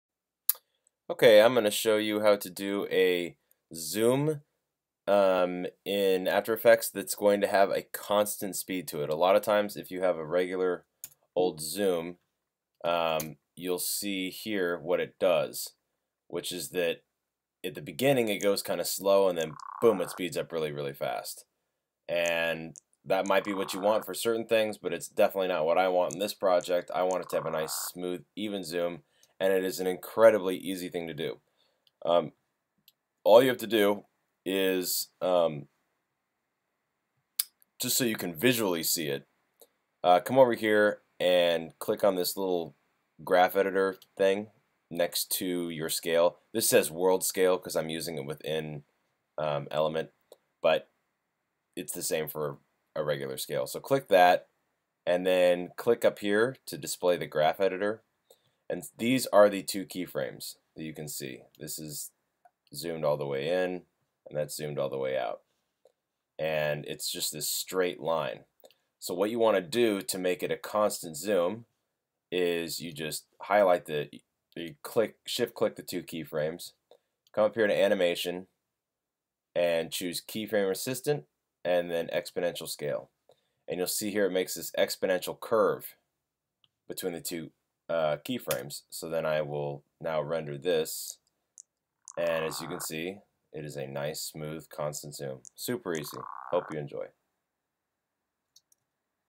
Smooth, Constant Zoom In Adobe After Effects
This is how to achieve a smooth, constant zoom over time in Adobe After Effects by utilizing the 'exponential scale' feature in keyframe assistant.